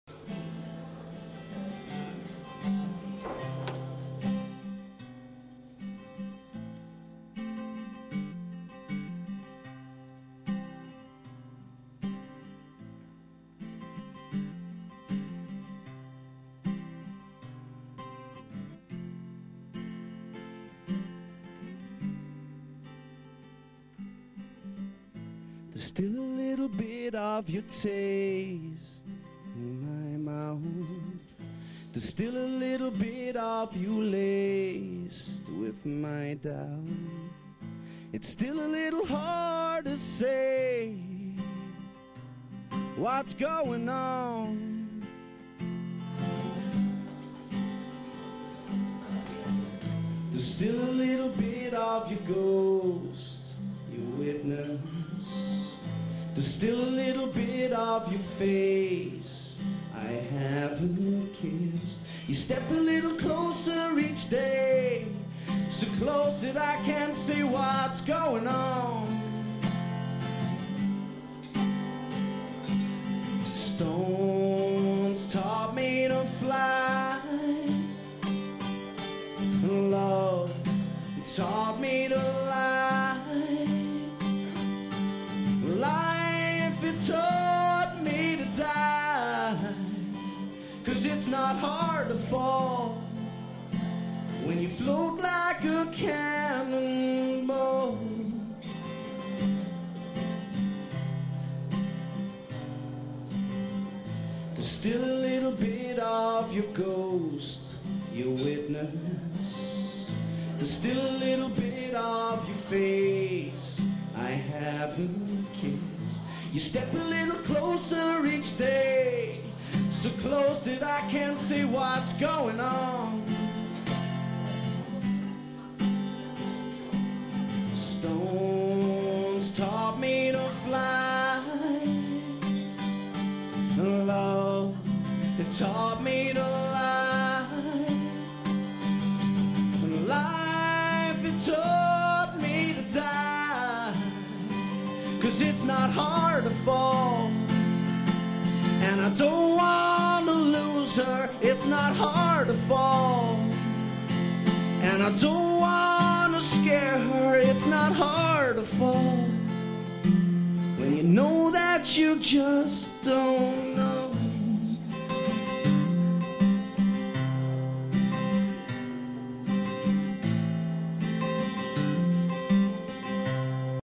Safc Foundation Fashion Strikes Andy Reid covers Damien Rice
Sunderland Footballer Andy Reid Sings for charity